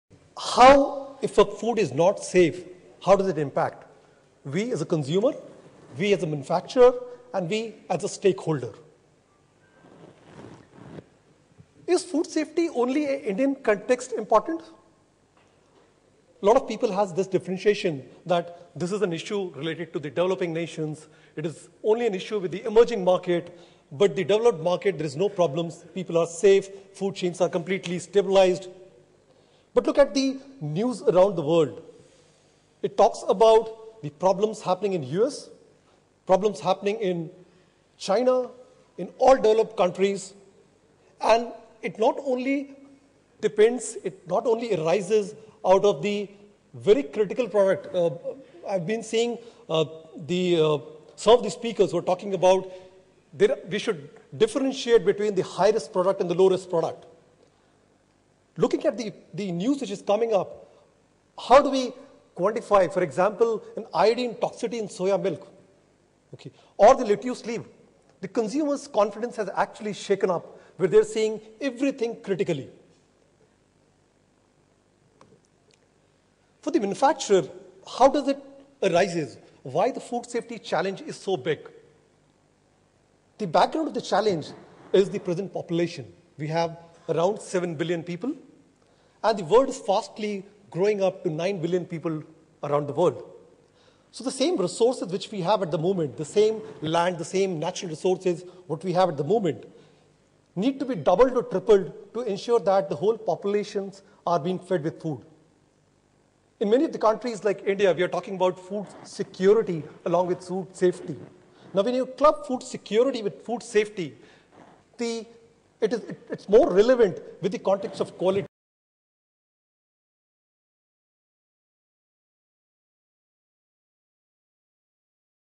Dr. Pankaj Jaiminy, AVP, TÜV SÜD South Asia
Dr. Pankaj Jaiminy extends a presentation on food safety, global food incidents and its impact at TÜV SÜD's Safe Future Now summit on food safety.